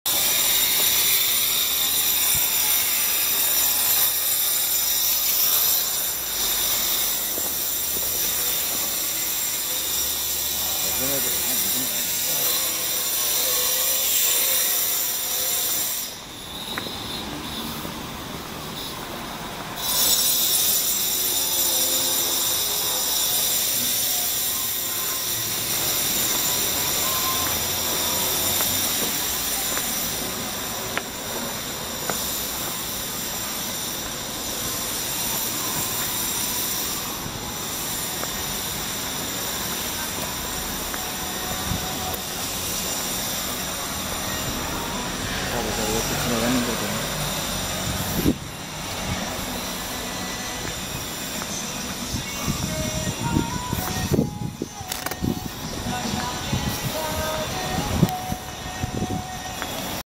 Jeonju Bibimbap's City , 전주 비빔밥의 도시 , 전주 효자동 신시가지. North Jeolla Prov,전라북도, 全羅北道 . Jeonju . KOREA
전주 송천동 신시가지. Jeonju Songcheondong .全州 松川洞 新都市. Jeonju. KOREA
4개의 중학교가 있으며 , 2개의 고등학교가 있으며, 시립도서관이 2개 있어서 교육의 환경이 잘 되어 있습니다.

이렇듯 전주 송천동(全州 松川洞)은 전라북도 전주에서 장래가 촉망되고 발전가능성이 아주 큰 " 전주 북부권 신도시" 입니다. 전주 덕진구 송천동 신도시 . North Jeolla Province( 전라북도 全羅北道 ) . Jeonju . KOREA